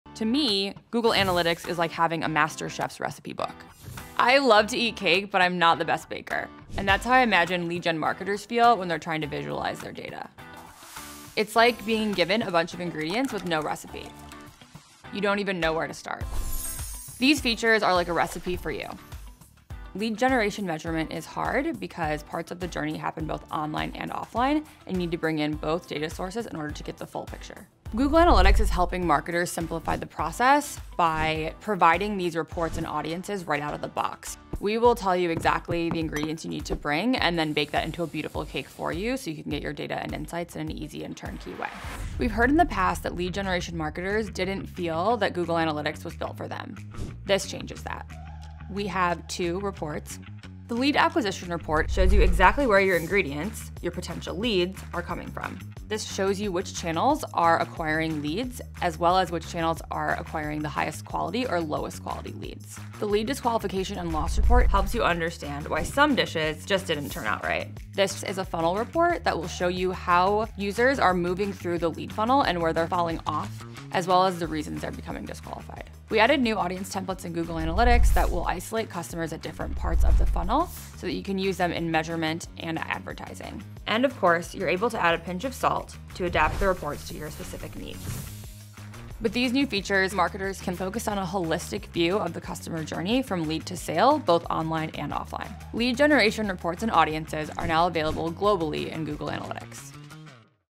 Lead generation reports in Google Analytics 2025 explained by Product Manager
Measuring and optimizing your lead generation campaigns shouldn’t require custom workarounds. We heard you; you need integrated, pre-built solutions.

Google Analytics now includes a new suite of lead generation default reports and suggested audiences. Hear how Carly Boddy, a Google Analytics Product Manager, explains this update as a ‘recipe for success.’

With these new tools, you can now:
→ See where potential leads are coming from, with the lead acquisition report
→ More easily understand why leads are falling off, with the lead disqualification and loss report, and
→ Quickly create new audience groups, with dedicated lead generation audience templates